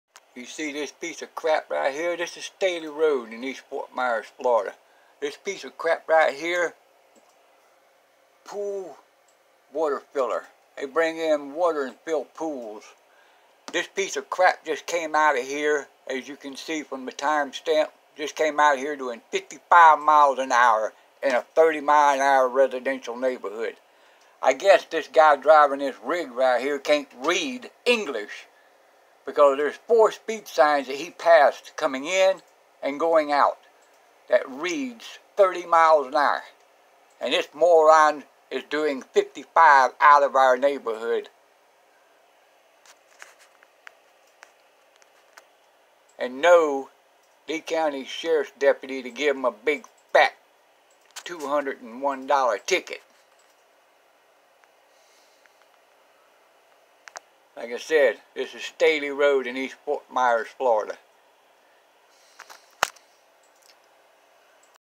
you can retire to Florida but this will be your neighborhood right here
NEIGHBORHOOD FROM HELL Staley rd  IN E.FORT.MYERS,FL.